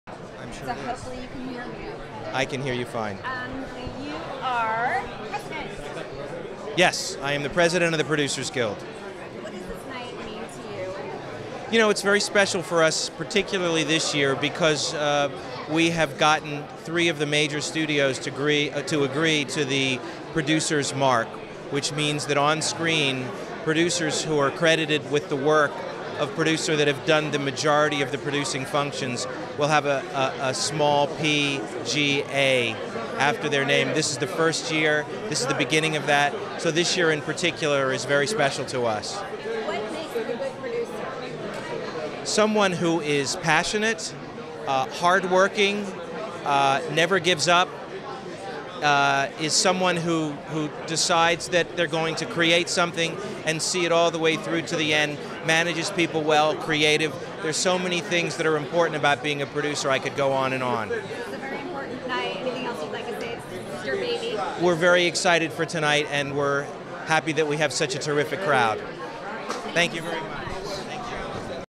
INTERVIEW - Mark Gordon on this years Producer Guild Awar...
INTERVIEW - Mark Gordon on this years Producer Guild Awards, on what makes a good producer at the 24th Annual Producers Guild of America Awards on 1/26/13 in Beverly Hills, CA .